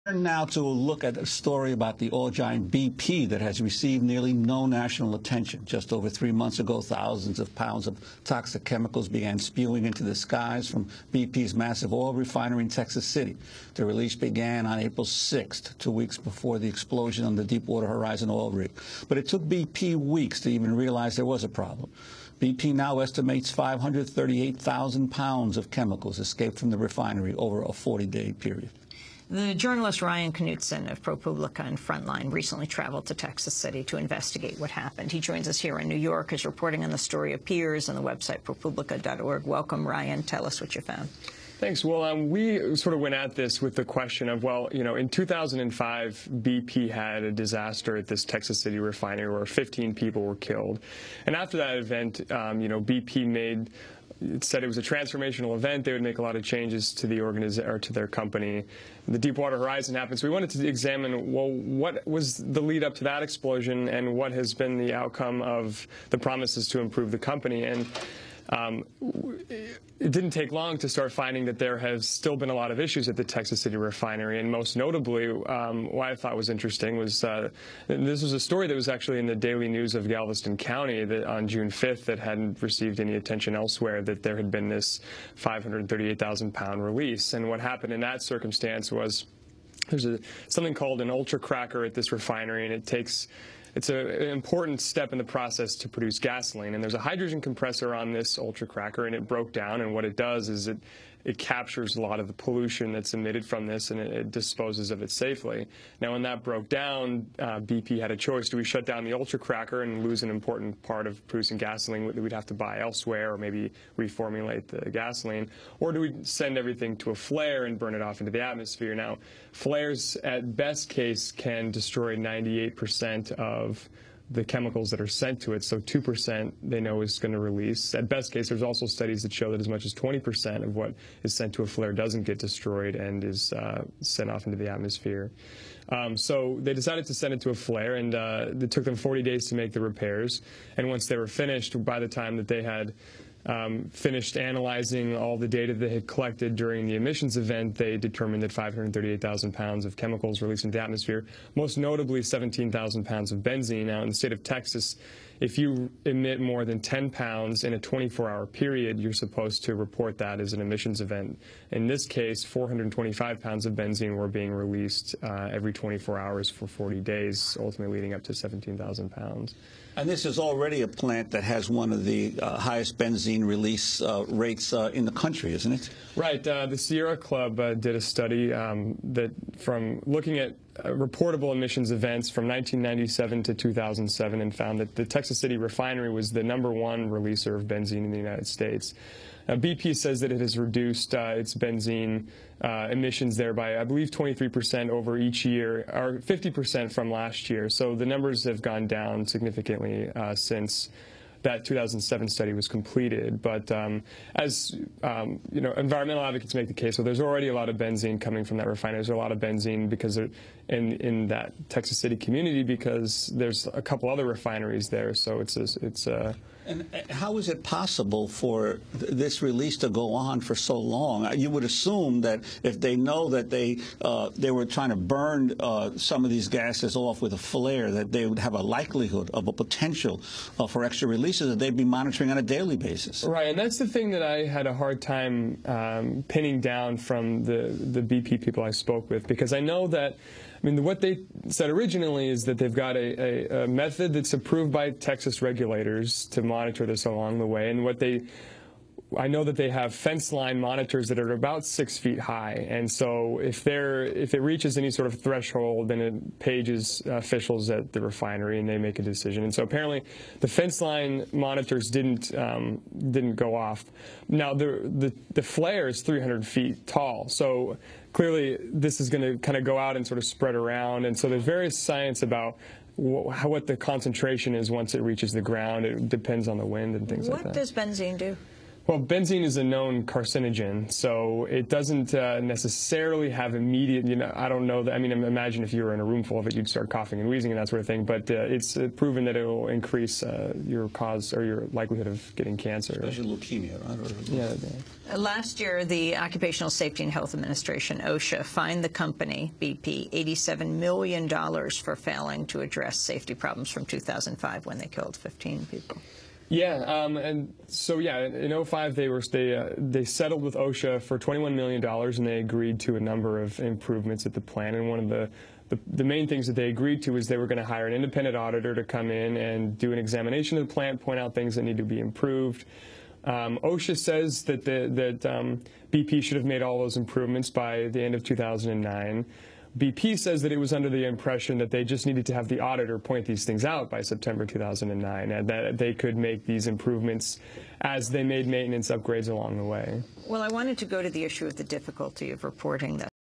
BP Texas Refinery Had Huge Toxic Release Just Before Gulf Blowout
ProPublica: BP Texas Refinery Had Huge Toxic Release Just Before Gulf Blowout